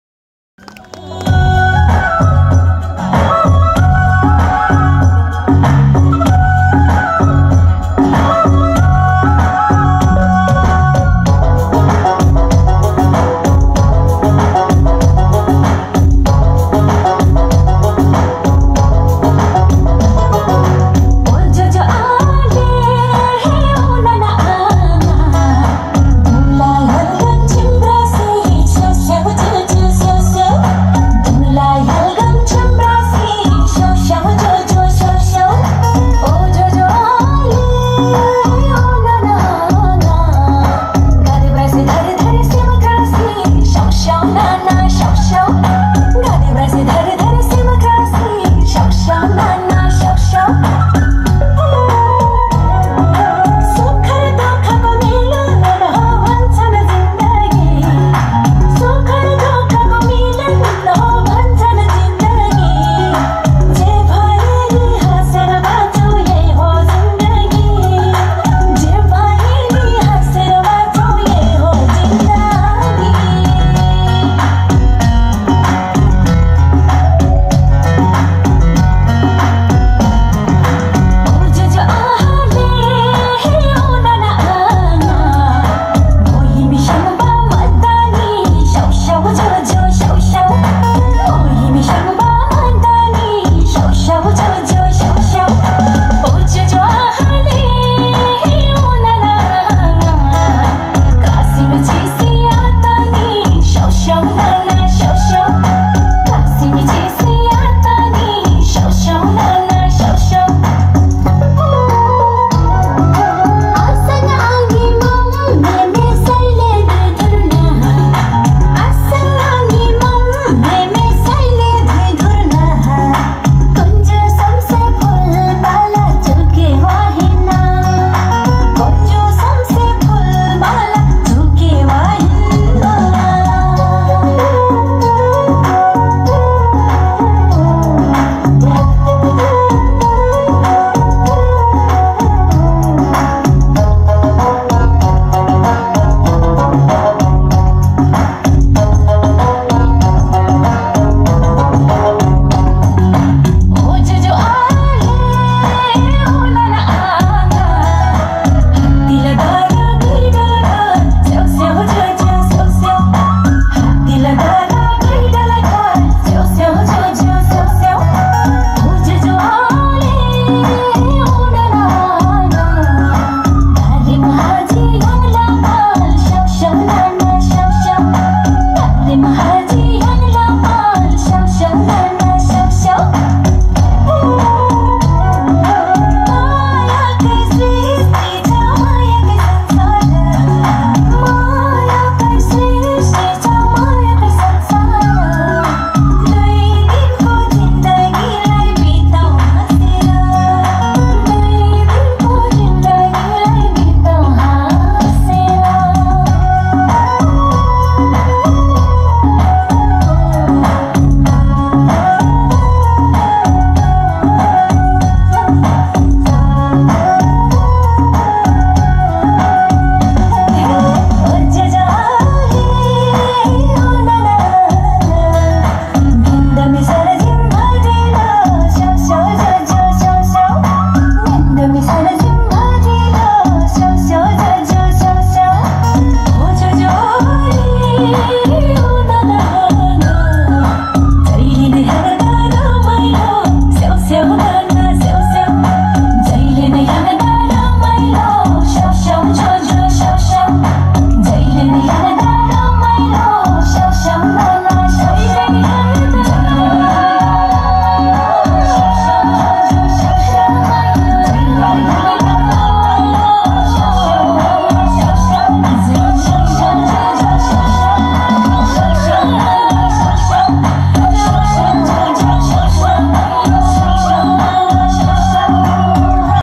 International art and cultural performances from Nepal
การแสดงศิลปะ วัฒนธรรม นานาชาติ  จากประเทศ เนปาล
ณ  บ้านแตล  อำเภอ ศรีขรภูมิ  จังหวัด สุรินทร์  ประเทศไทย
At Ban Tael, Sikhoraphum District, Surin Province, Thailand
  Walk the fashion show floor, traditional silk dress Surin City / Ethnic groups / International 15 countries / International network partners in education, art and cultural exchange
Video source / Yasothara Siripapraphakorn